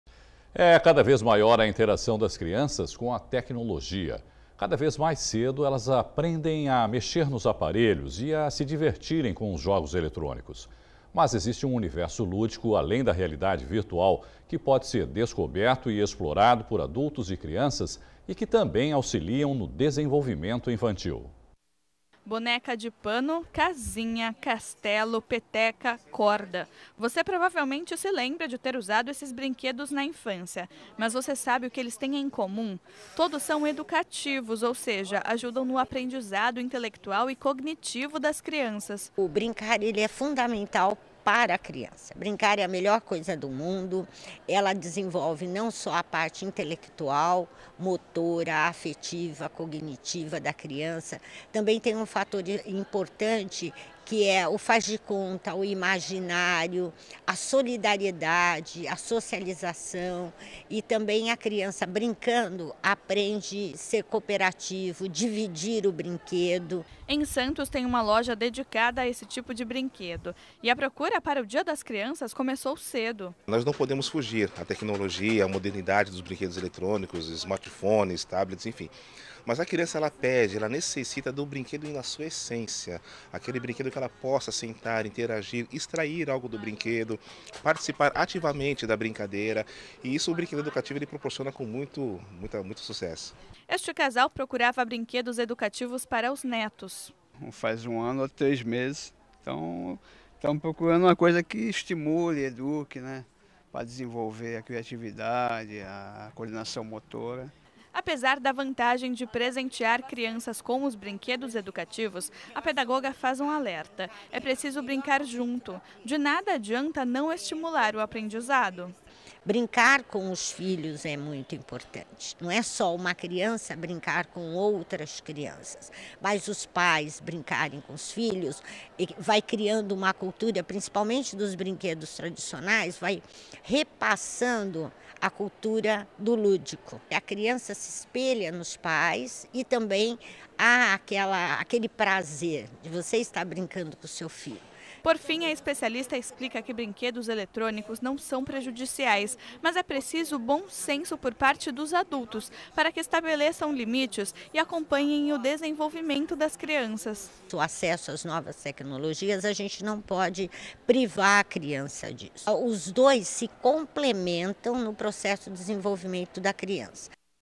Brinquedos educativos além de divertir auxiliam no desenvolvimento
É cada vez maior a interação das crianças com a tecnologia, mas existe um universo lúdico além da realidade virtual que pode ser descoberto e explorado por adultos e crianças, e que também auxiliam no desenvolvimento infantil.